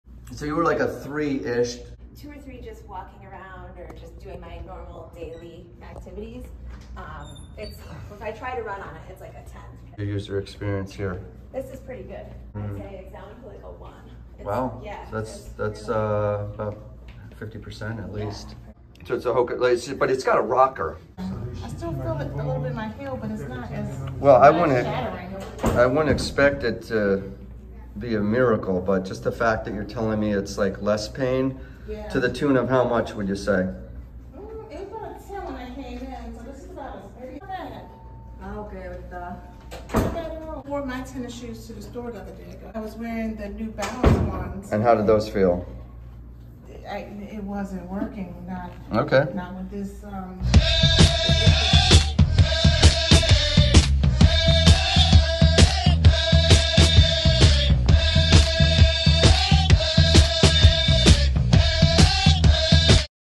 6/6 Walked Out w/ Less Heel Pain- New Podiatry Paradigm
Dr. Pearl is the 1st Clinician in the World to Embrace the idea of Using the Antepes Training Shoe as an intervention for injury. The Shoe is designed designed specifically to Make the heel off phase occur earlier and activate the gastrocnemius and peroneal muscles.